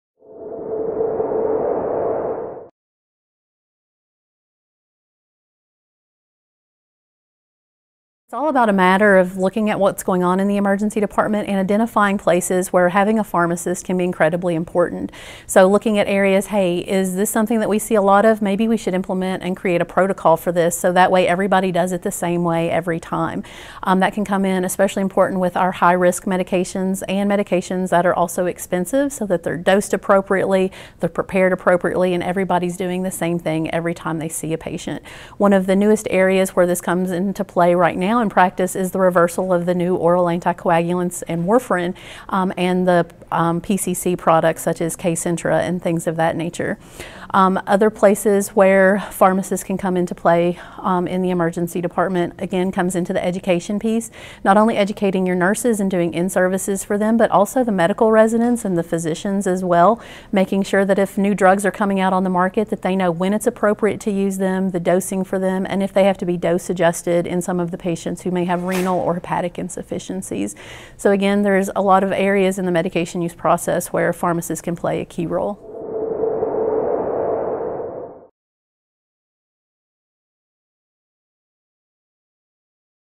Strategies Pharmacists Can Implement to Decrease Medication Errors
Stephanie Baker Justice, PharmD, BCPS, a pharmacy specialist in the emergency department at Charleston Area Medical Center, discusses strategies pharmacists can implement to decrease medication errors.

This interview was recorded at the ASHP 2014 Midyear Clinical Meeting in Anaheim, California.